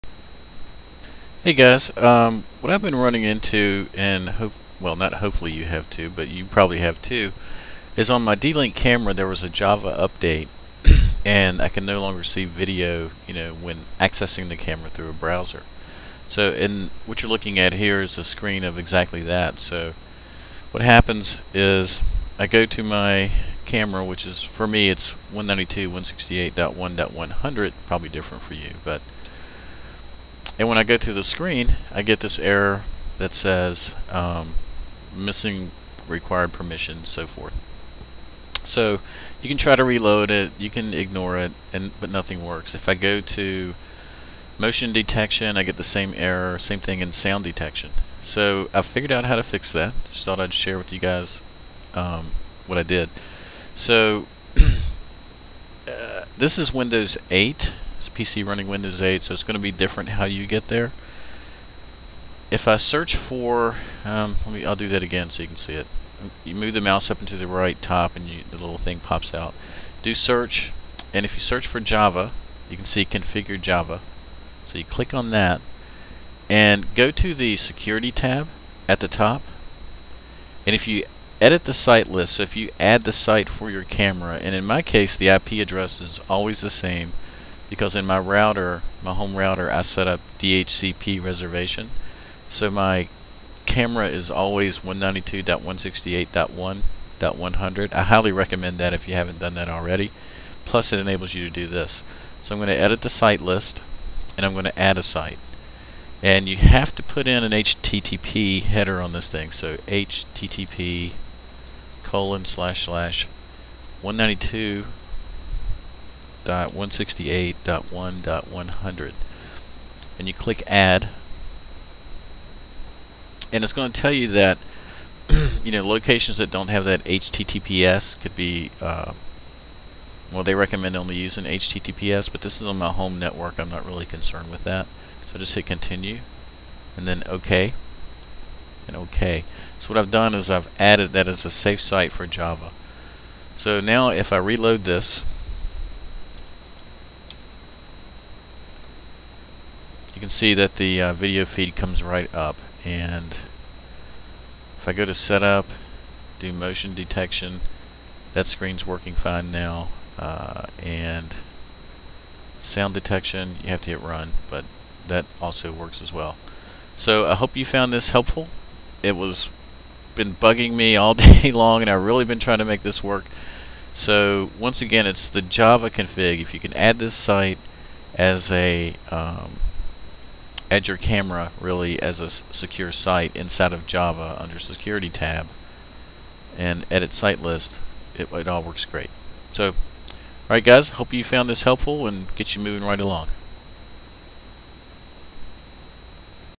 DLink Camera Java Permissions Error
How to fix or work around the java errors on your browser when using the dlink cameras.  My camera model in this case is a DCS-5020L but this may work for most since the issue is the java update.